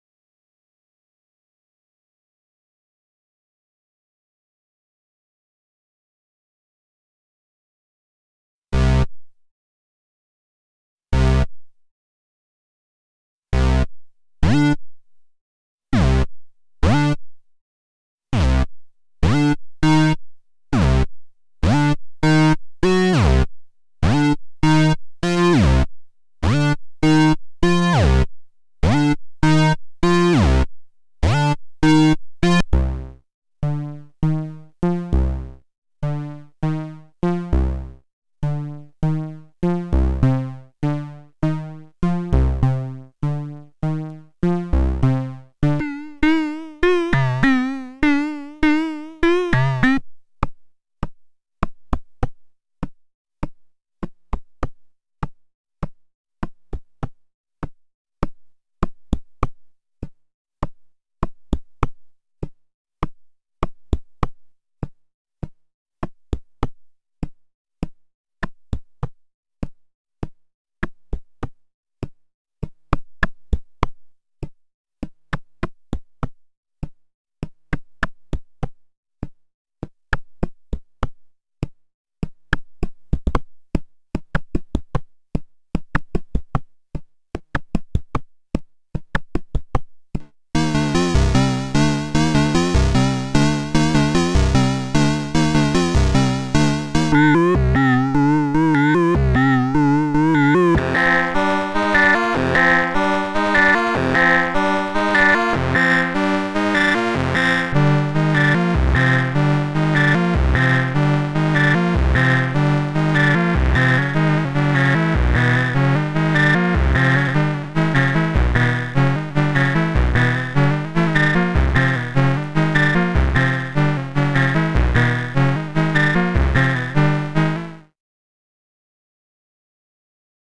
Edit a synth with a browser: MeeBlip meets the Web
Design sounds, try them out, and share them with this free Web tool created by Ben Schmaus (Factuomo).

Open source hardware with open source Web tools!